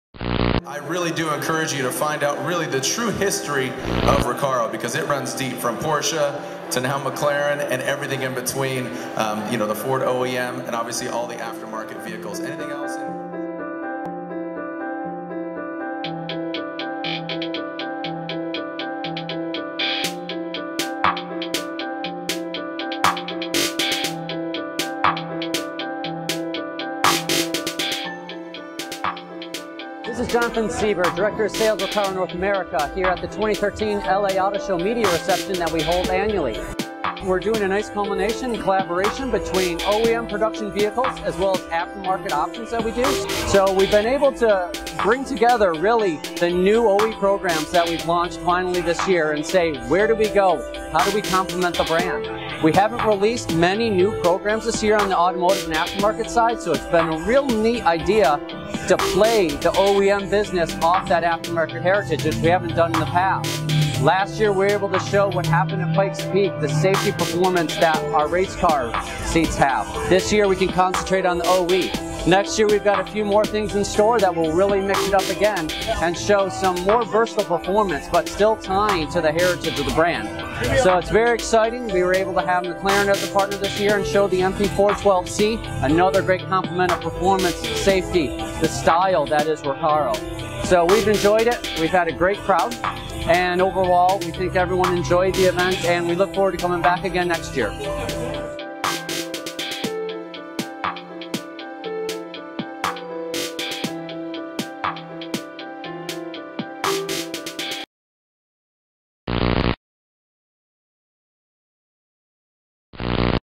RECARO 2013 LA Auto Show Event
On November 20th, the ID Agency held a reception for RECARO Automotive on the first night of the Los Angeles Auto Show, inviting enthusiast and mainstream media, OEM representatives, tuning shops, aftermarket suppliers, tire manufacturers and automotive partners to meet the RECARO team.

RECARO wanted to highlight its OEM programs in the United States along with its aftermarket programs featuring selection of project cars that came straight from SEMA.

On display from the OEM world were the McLaren 12c, the Ford Focus ST and Fiesta ST plus a Camaro ZL1 -- all of which feature RECARO seating.

Project cars on display at the event included Ken Block's Ford TrackSTer, Vaughn Gittin Jr's RTR Mustang, Mister Cartoon's Special Edition ZL1 Camaro, Bulletproof Automotive SCION FR-S, LTMW BMW M3 with Liberty Walk kit, LTMW Subaru BRZ, John Pangilinan's Hyundai Genesis, Modified Magazines Chevy Sonic, D3′s modified CTS, HG Motorsports Mercedes-Benz CLA250, and GI Motorsports AMG Black Series.

Notable attendees include world renowned drivers Tanner Foust, Bucky Lasek, Daijiro Yoshihara and Tommy Kendall plus pro athletes CJ Wilson and Sal Barbier as well as representatives from Ford Motor Company.

Thanks to our generous sponsors, including McLaren, Icelandic Glacial and Red Bull.